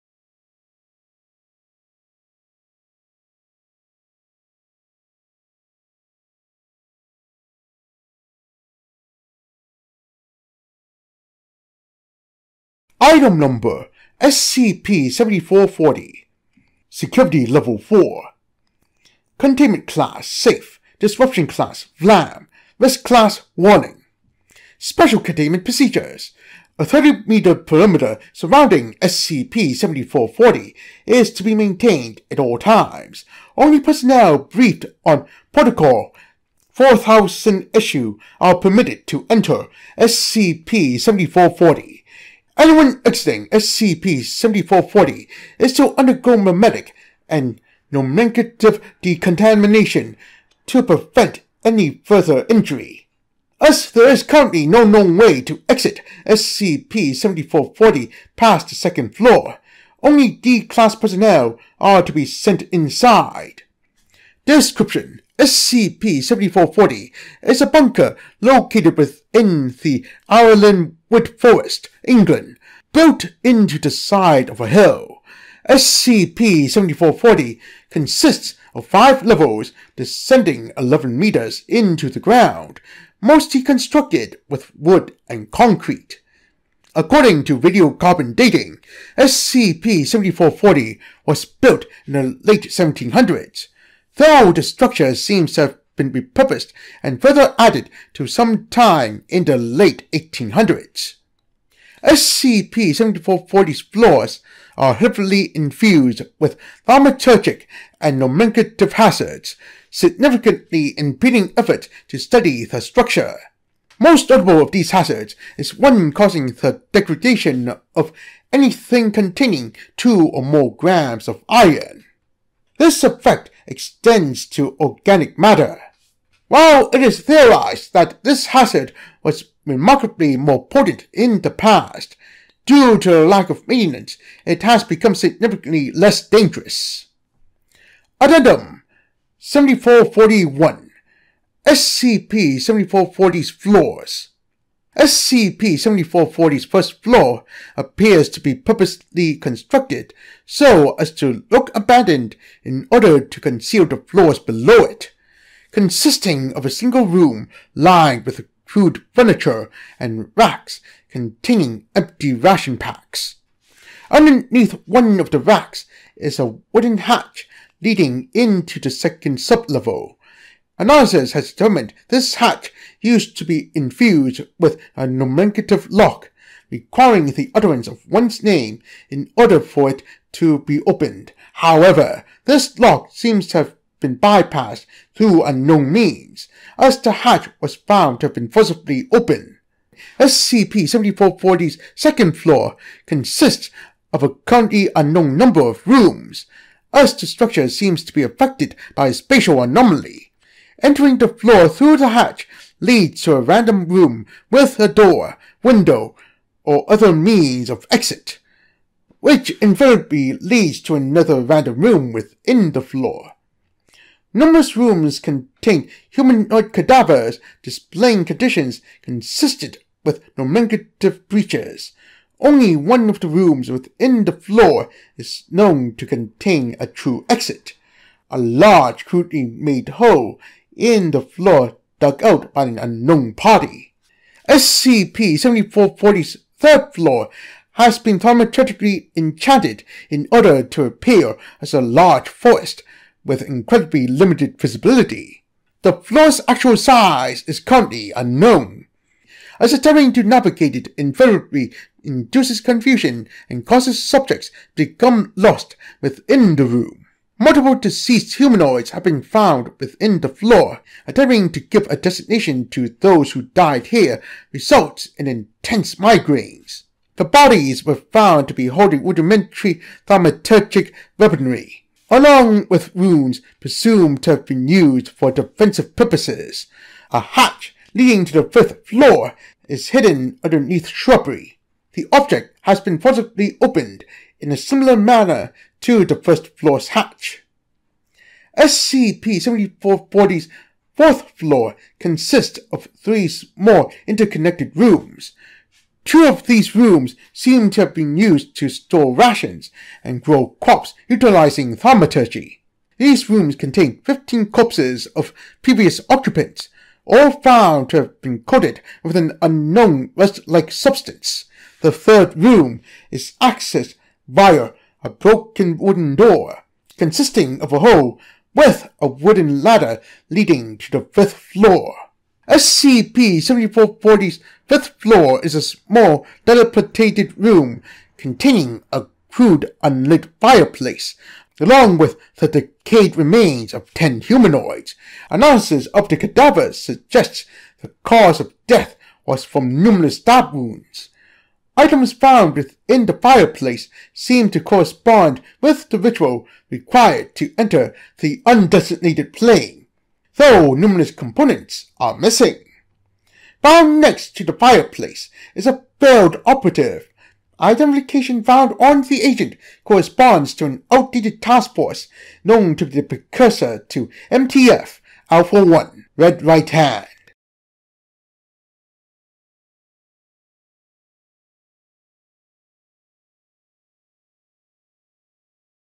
SCP 7440 Nameless Bunker| Fair Folk-related SCP
SCP-7440 is the titular bunker with five floors that are traps with name-stealing properties, as well as having rooms that confuses invaders and an anti-iron trap.